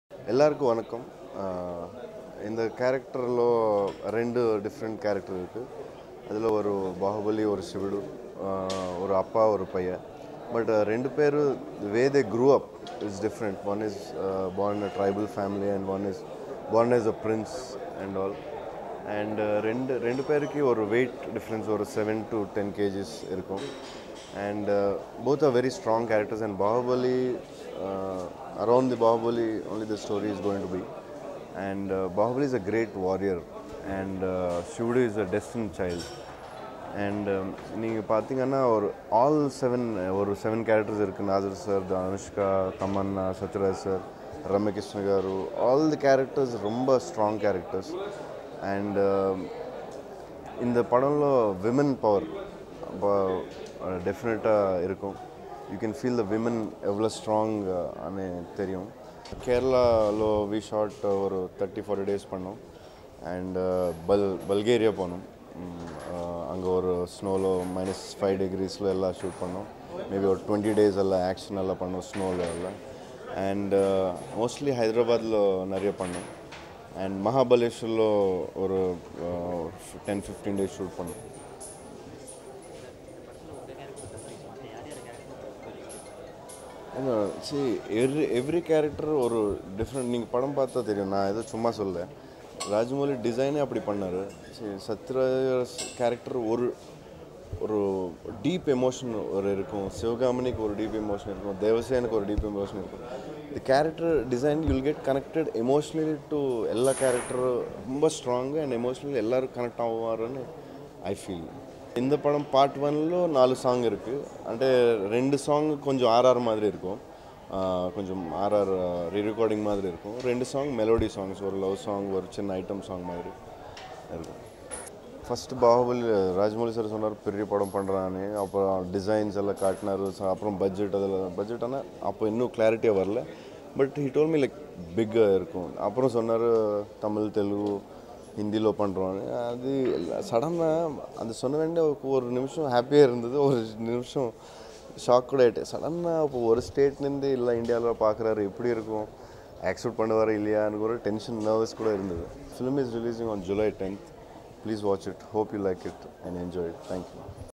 Actor Prabhas Exclusive Interview Baahubali movie on Liveonwoods
Prabhas Raju Uppalapati (born 23 October 1979),[1] popularly known as Prabhas, is an Indian film actor working in Telugu cinema. He is notable for his roles in Varsham, Chatrapathi, Billa and Mirchi. His upcoming film Baahubali is touted to be the most expensive undertaking in the history of Indian cinema.[2] in which he is playing dual role.